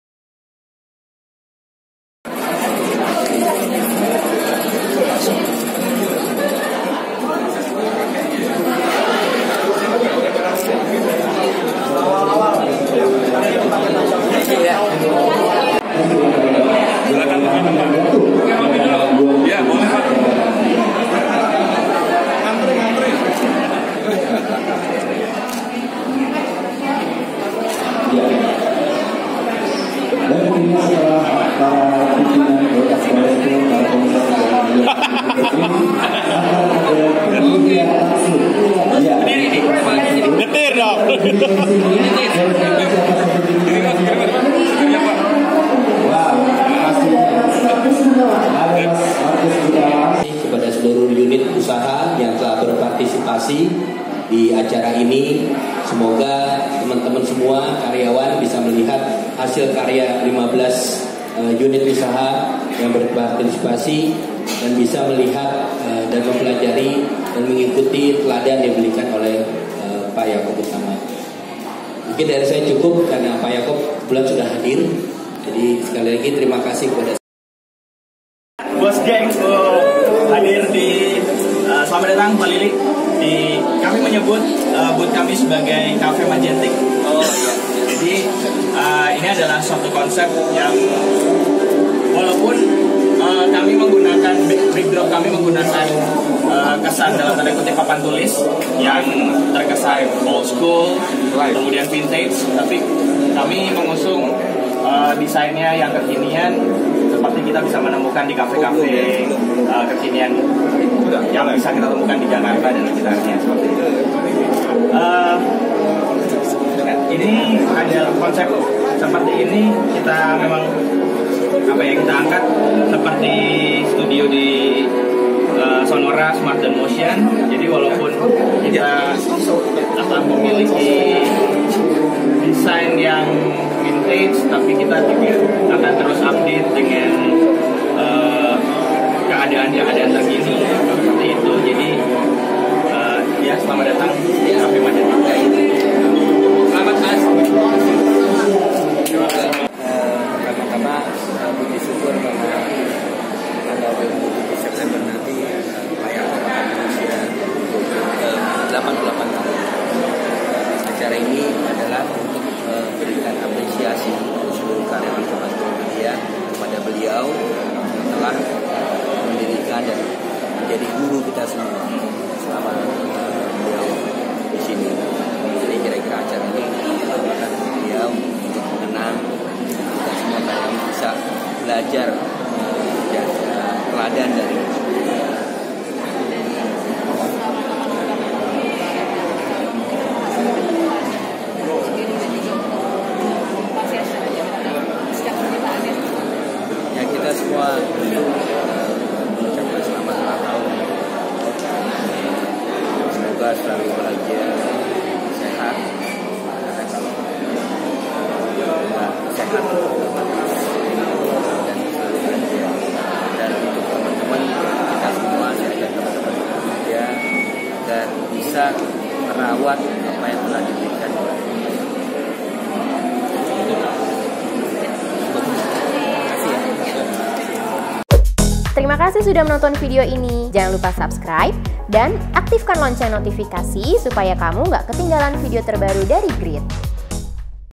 Semarak Kreasi Dan Inovasi Karya Seni Rayakan Ulang Tahun Pendiri Kompas Group
Grid.ID - Semarak Kreasi Dan Inovasi Karya Seni Rayakan Hari Jadi Pendiri Kompas Group

Pendiri perusahaan media Kompas Gramedia, Jakob Oetama. yang ke 88 tahun dirayakan secara semarak dan meriah. yang mana dalam acara tersebut turut menghadirkan berbagai kemeriahan berbagai kreasi seni baik foto hingga karikatur yang menarik.

0:50 Lilik Oetama memberikan sambutan
1:20 Lilik Oetama mampir di booth Motion Radio
3:15 Harapan Lilik Oetama di ulangtahun Jakob Oetama yang ke 87 tahun

EDITORIAL OFFICE

ADVERTISING